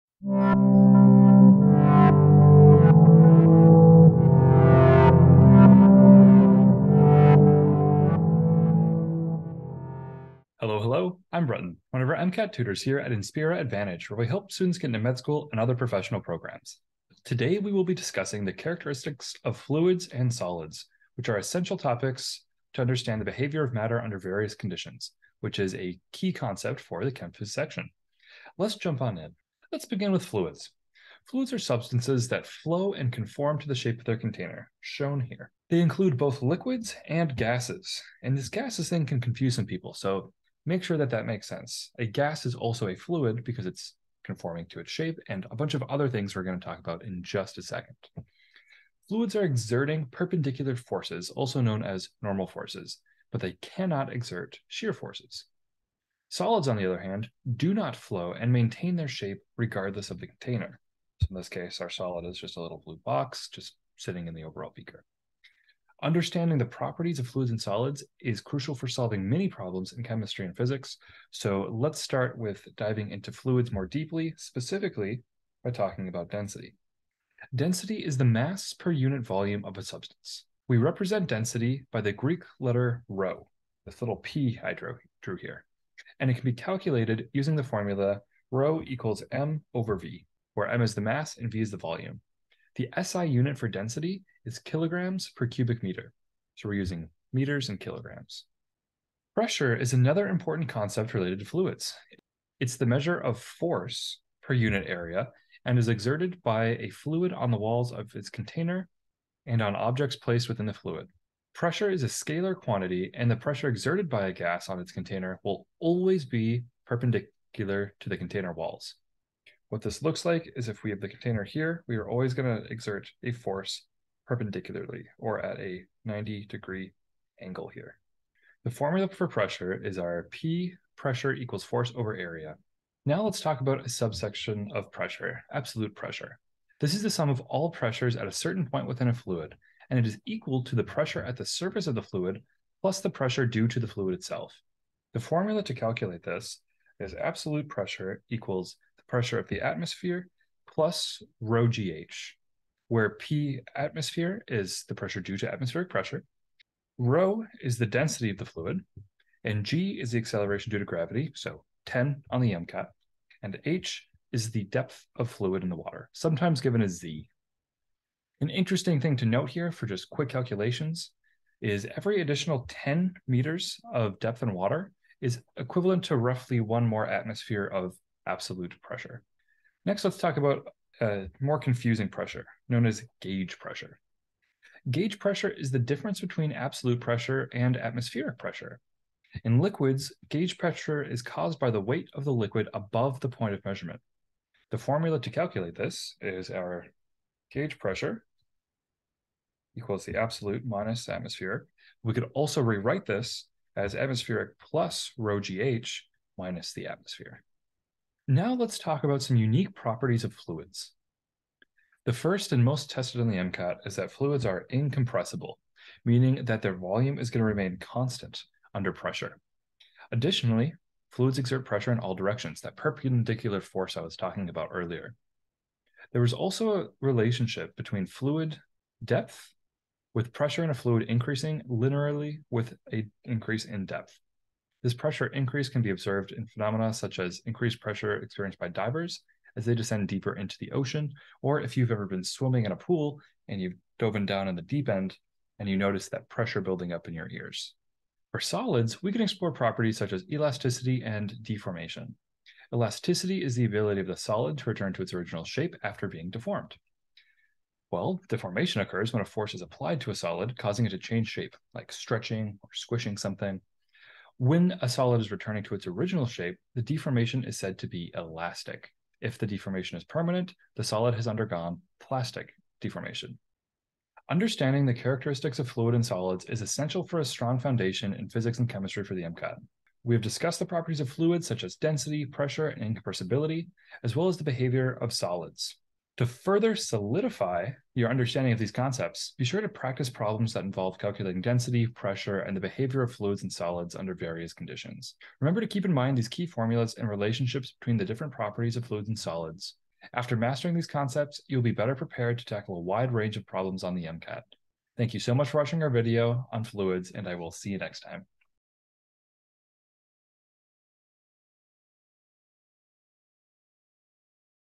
Physics: Fluids & Solids | MCAT Crash Course
Explore Physics: Fluids & Solids for the MCAT in this MCAT crash course! Follow along as Bretton, one of our 99th-percentile MCAT tutors, breaks down this fundamental concept to set you up for success on the MCAT!

Welcome to today's video where we're unraveling the significance of Physics - Fluids & Solids in the context of the MCAT. These fundamental concepts go beyond textbook physics; they form the basis of understanding critical biological and physiological processes within the human body. By mastering the principles of fluid dynamics, buoyancy, and solid mechanics, you're not only enhancing your problem-solving skills but also gaining a profound insight into the mechanics of circulation, respiration, and other vital bodily functions. Join us as we delve into this pivotal MCAT topic, equipping you with the knowledge and strategies necessary to tackle even the most challenging questions with confidence.